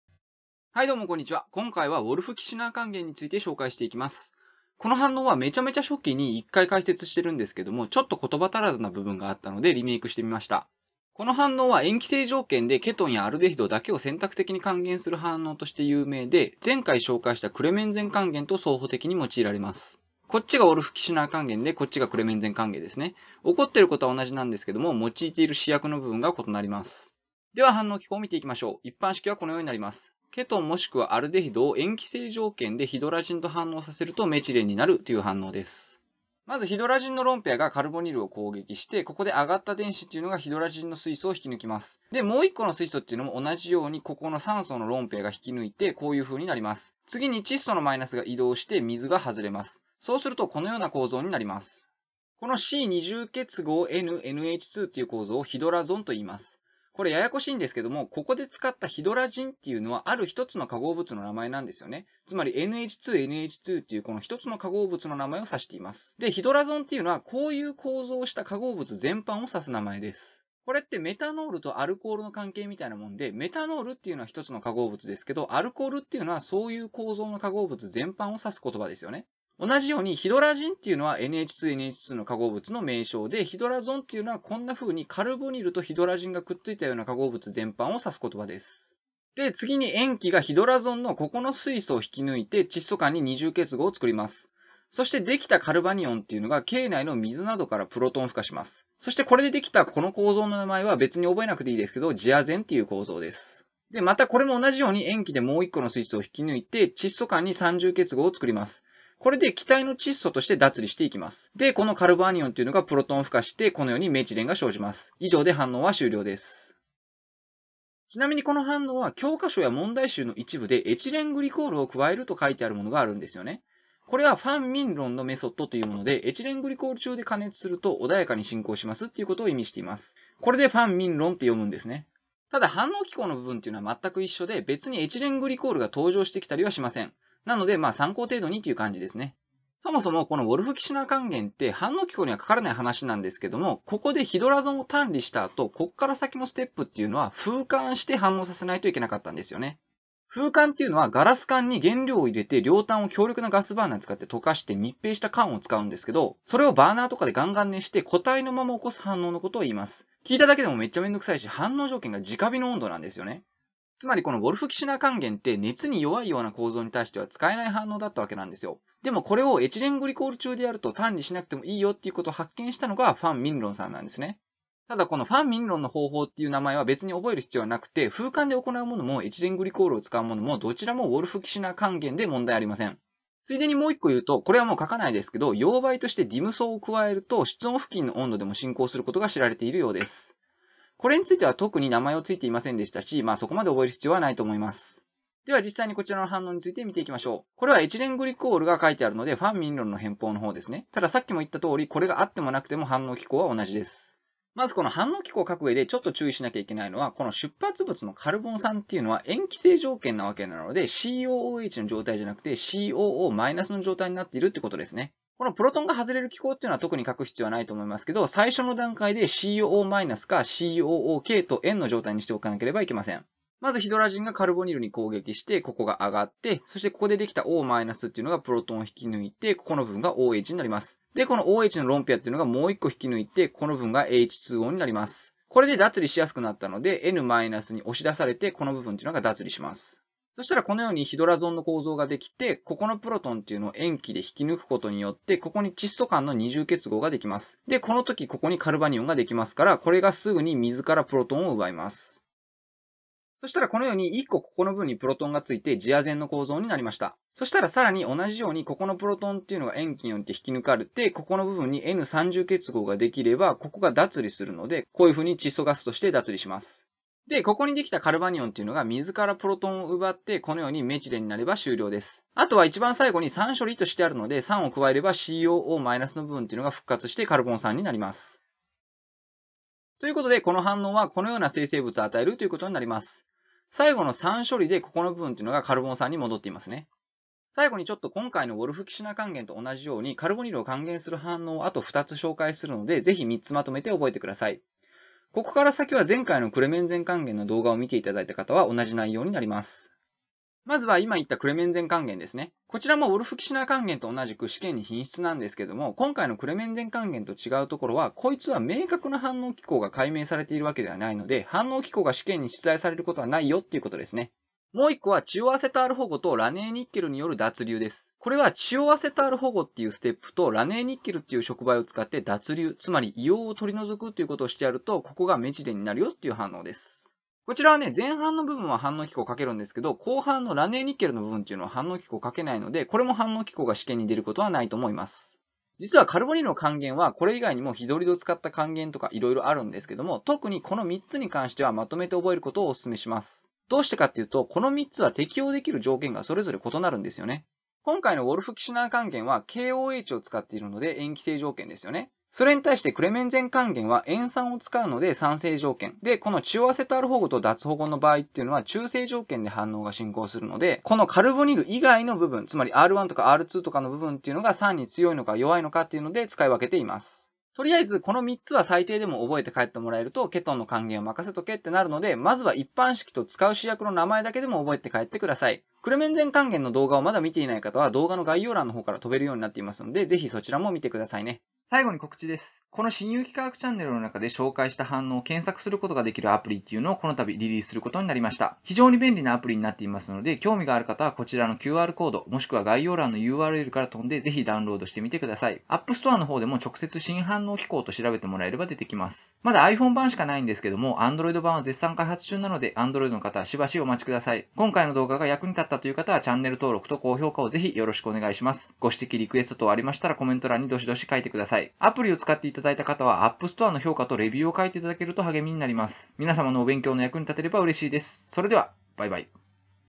ウォルフ・キッシュナー還元(Wolff-Kishner reduction)の反応機構【カルボニルの還元】
今回はウォルフ・キッシュナー還元(Wolff-Kishner reduction)について解説しています。

その他動画作成に用いたソフト
ChemDraw
Microsoft PowerPoint

ケトン/アルデヒド/還元/ヒドラジン/ヒドラゾン/ジアゼン/RaneyNi/ラニーニッケル/pH/アマルガム/亜鉛アマルガム塩酸/黄鳴竜の変法/ファンミンロンの変法